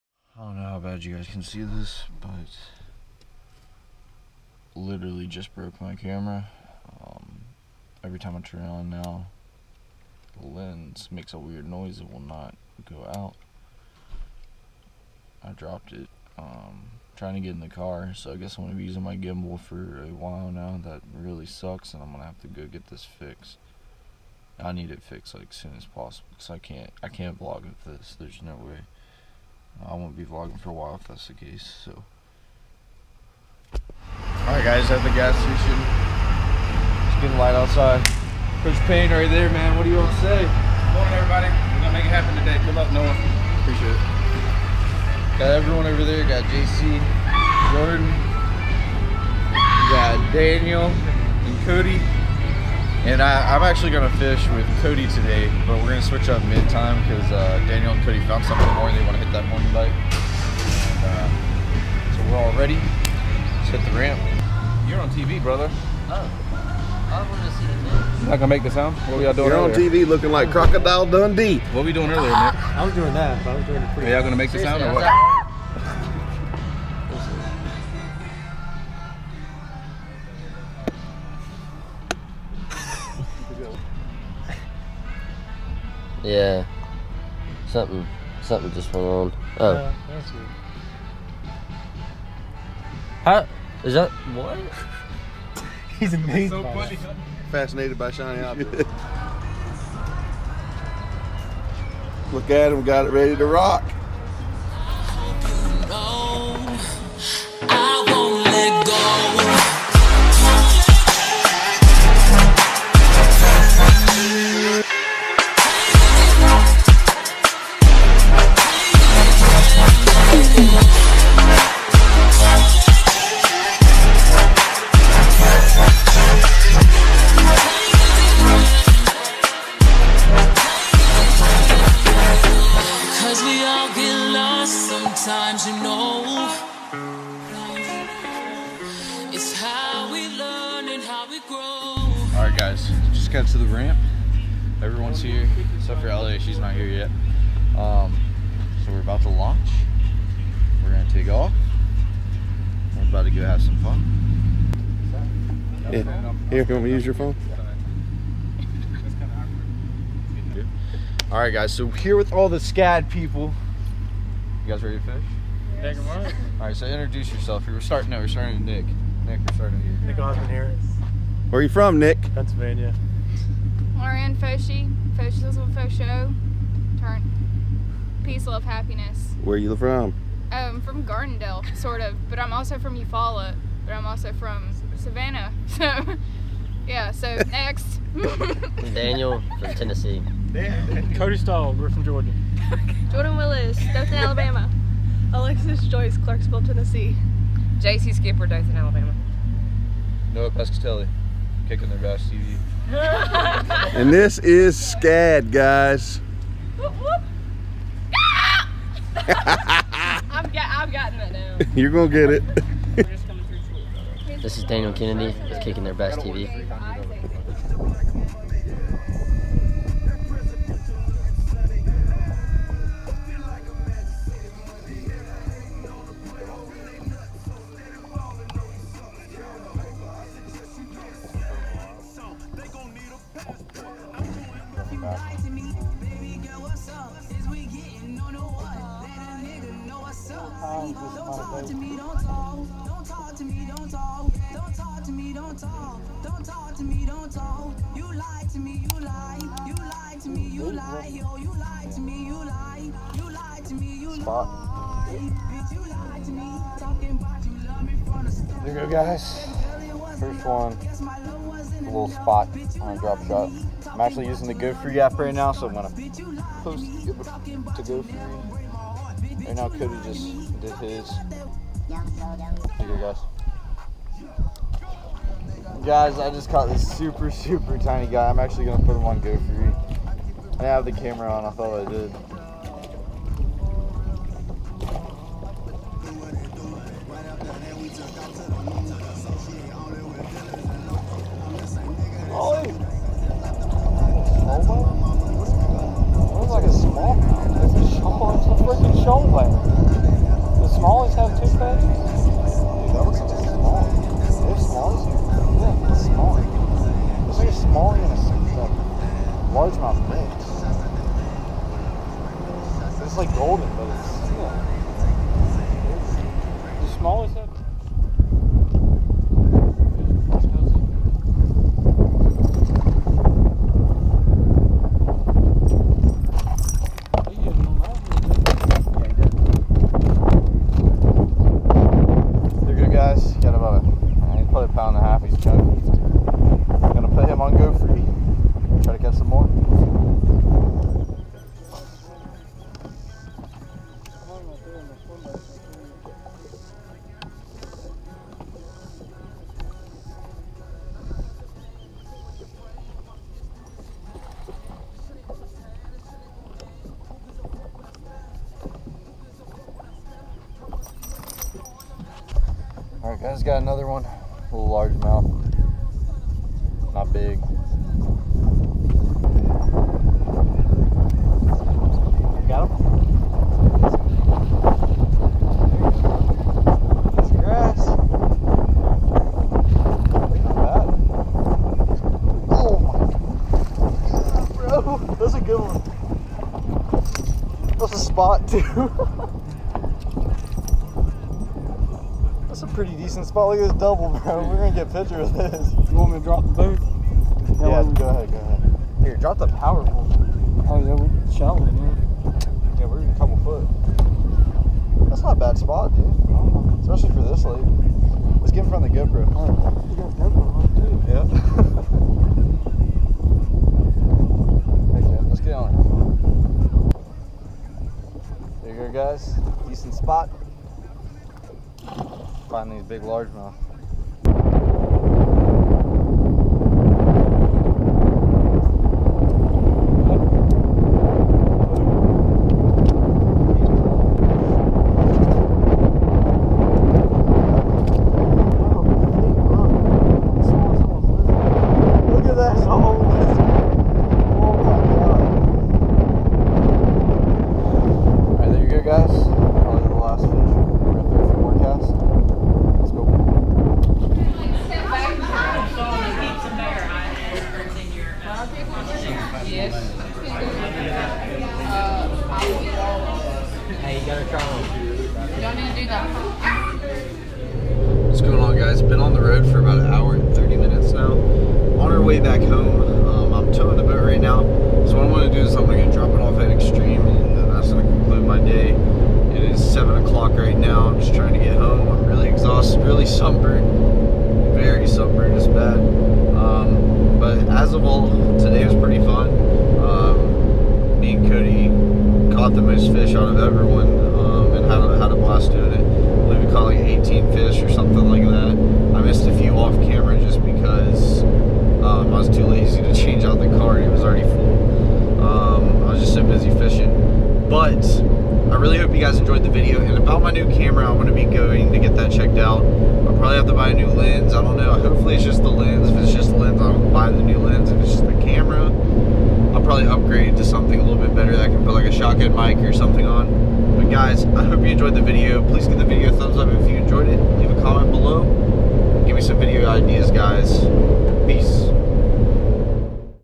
Catching Big Summer Bass Fishing - Lake Chickamauga ~ Day 3/3
Yooo KickinTheirBassTV back at it for the last day at Lake Chickamauga! The morning started off rough, as I was getting into the truck this morning We switched up our fishing partner today, we were able to catch some nice big bass and had a blast. Although many of our catches today were little bass. We finished this trip of with a dinner with everyone! I hope you all enjoyed the videos of this trip.
Do you have any Ideas for a video you would like me to do?